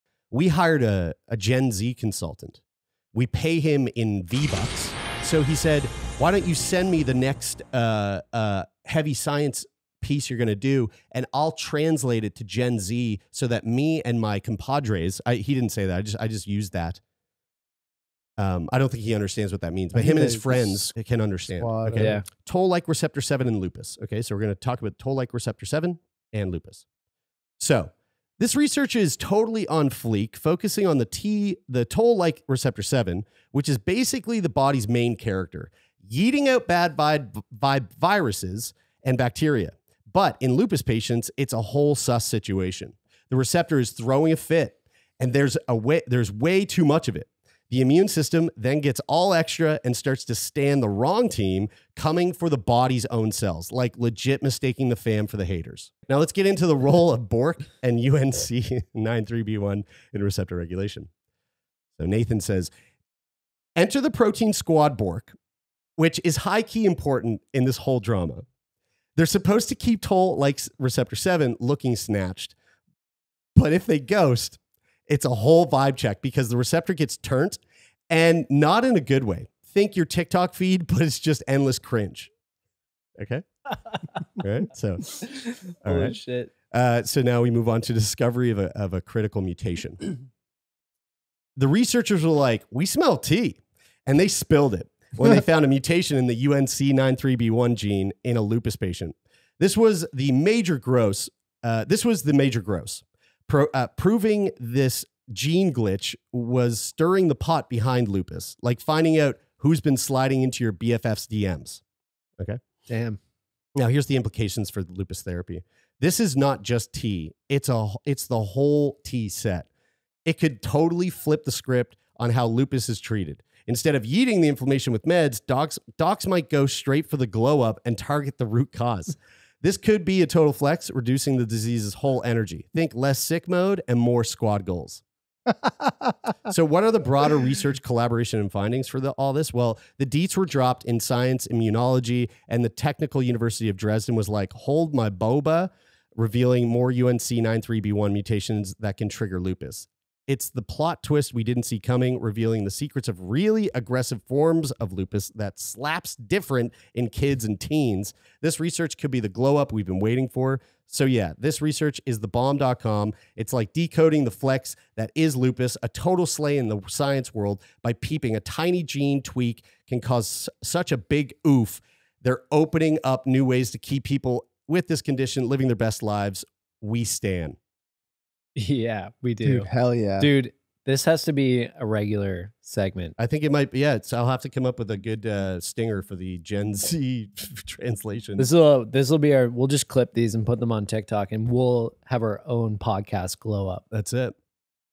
Millennials Hire Gen-Z Consultant for Podcast
Yo, Sickboy fam, we're diving into the science scene with a major key alert 🚨! Just dropped our first vid spilling the tea on a ground-breaking lupus discovery. It's straight fire, no cap! 🧬🔥 We're decoding the deets with all the Gen-Z drip, making it extra AF. Stay woke, we're just getting started.

^ That's good, right?